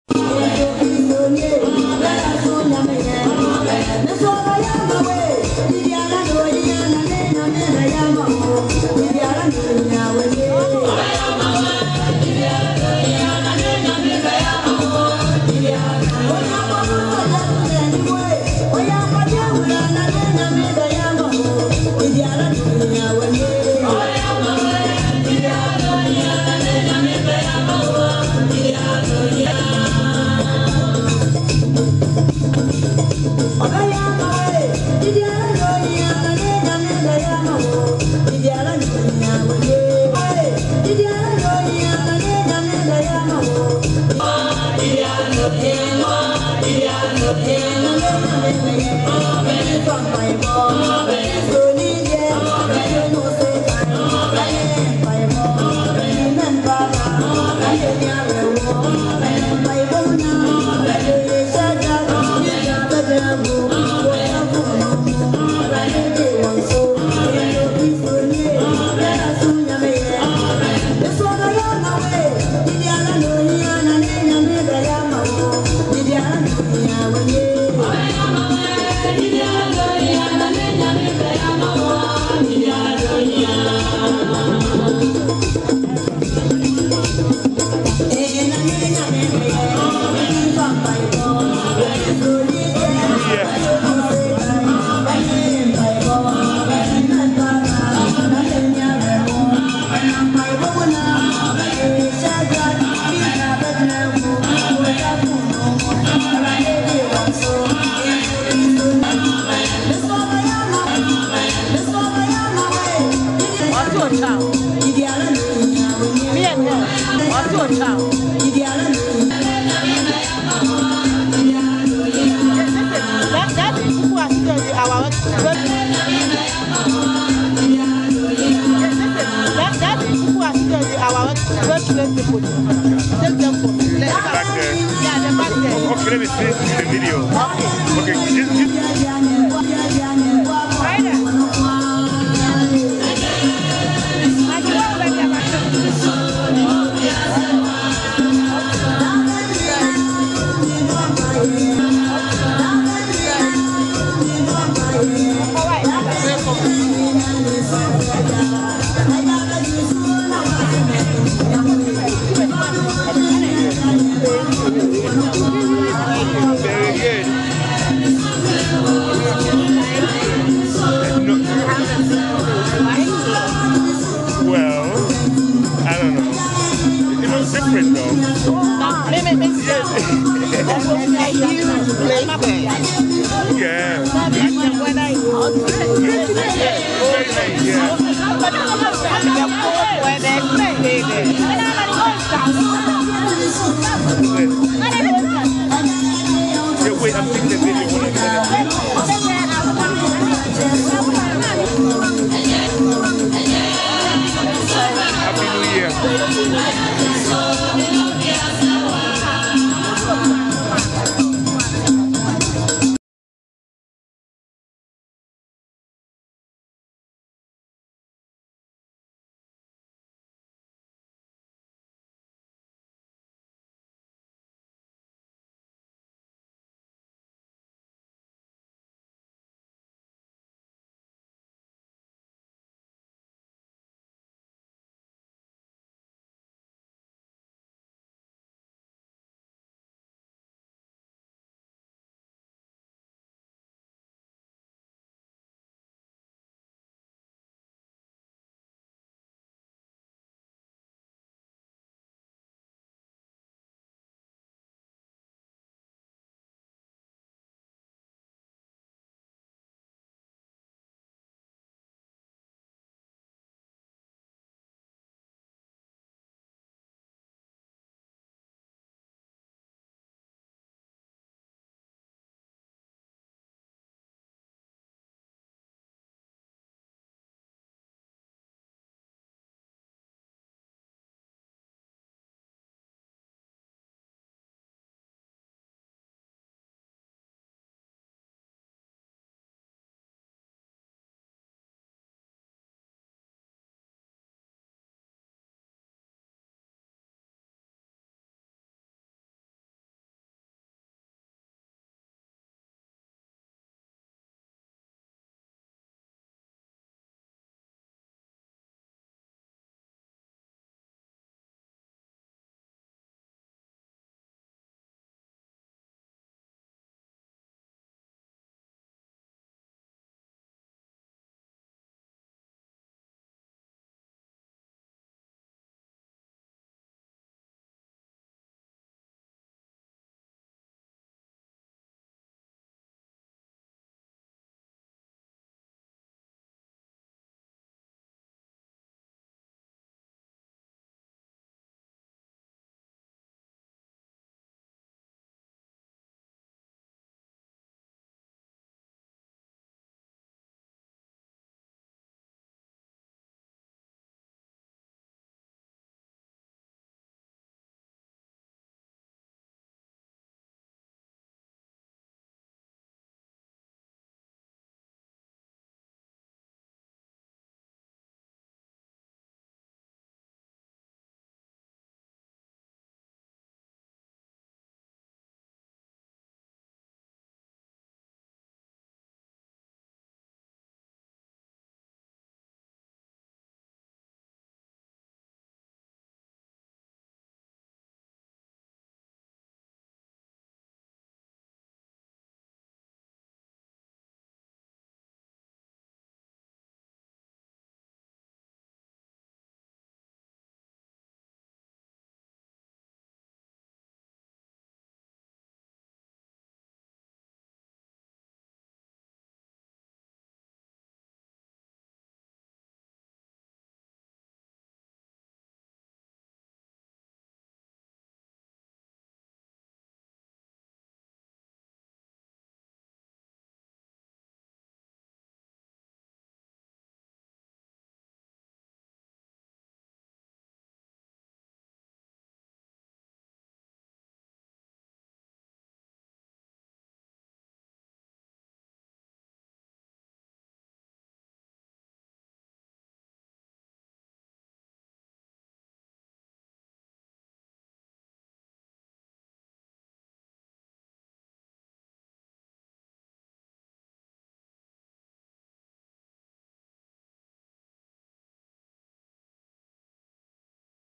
KOKOROKOO - Ghana In Toronto - Joe Amas's Life Celebration
KOKOROKOO - Ghana In Toronto - January 12, 2013 - Apostle's Continuation Church - Belfield. - Family, friends, co-workers and well-wishers came to support Amas's widow who expressed sorrow. Life is really short. Amas died in a car crash between Togo and Ghana. May His Soul Rest In Peace